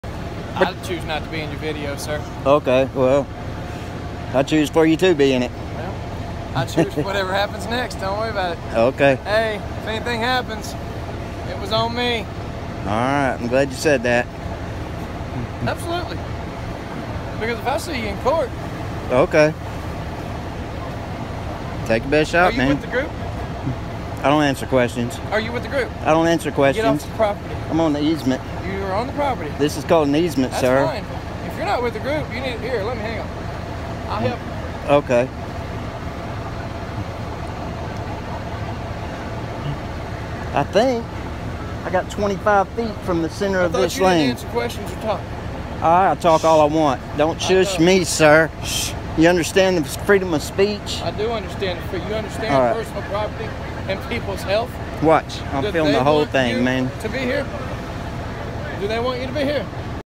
Call Whine One One!!!
Adam is used to barking orders at little kids on the skate floor!  I guess he thinks he can also do it out in public too!
$Bearandivy1